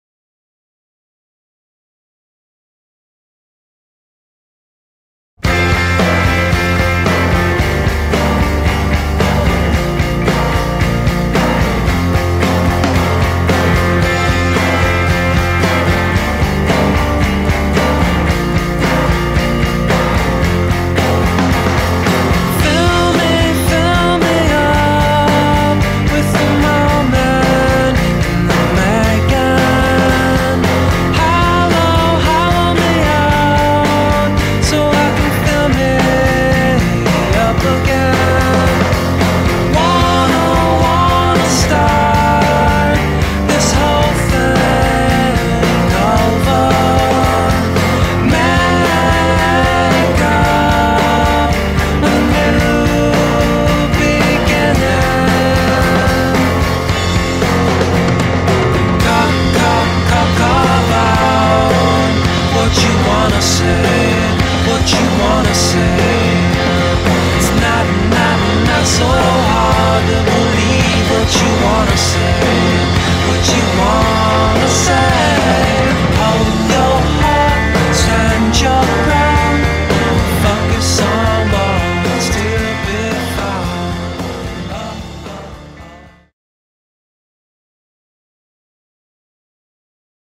Slideshow Workshop "Promove a tua marca pessoal no YouTube"
Evento realizado na Cidade das Profissões, Porto, 12 de Novembro de 2014.